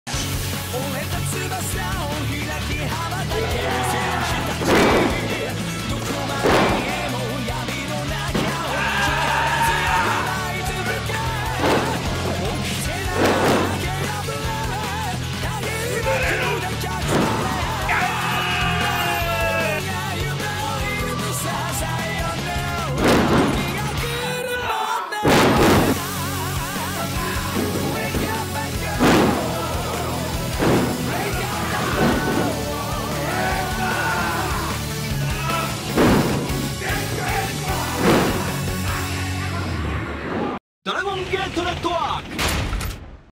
KING OF GATE 2020 配信告知
毎年恒例のドラゴンゲート・シングルNo.1決定戦『KING OF GATE 2020』を「DRAGONGATE NETWORK」にて全試合を独占配信致します！！